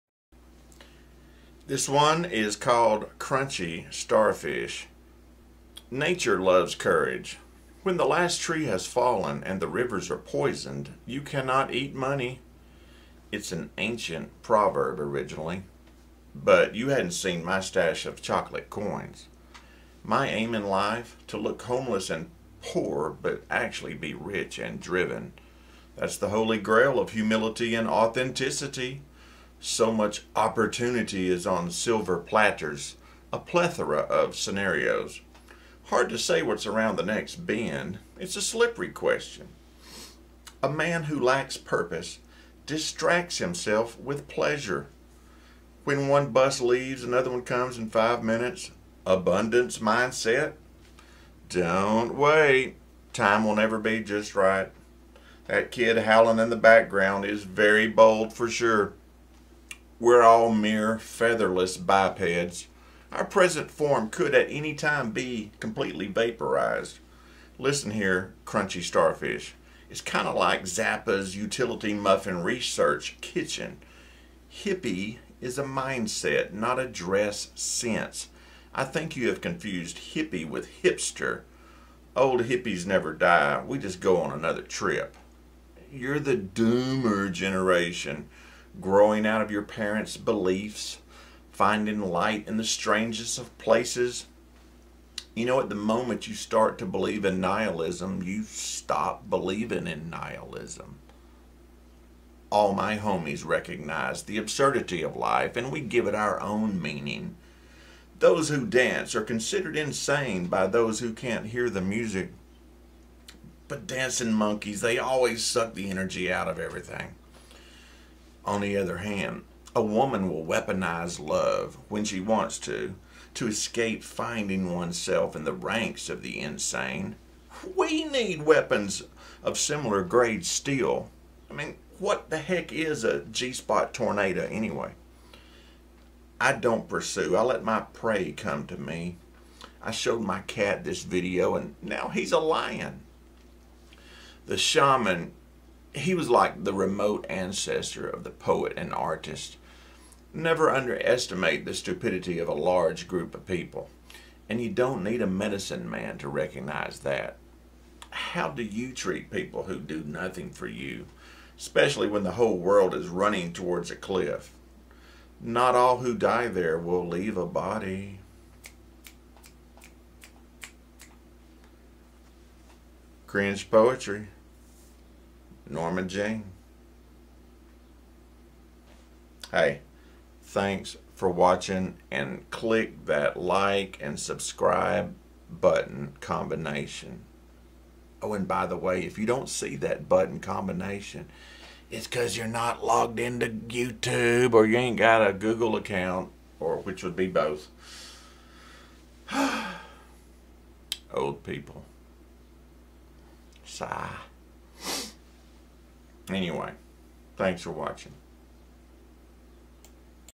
Crunchy Starfish! || Spoken Word Poetry
Crunch crunch...  lost a leg, now a crossfish... spoken word poetry by Lance Bailey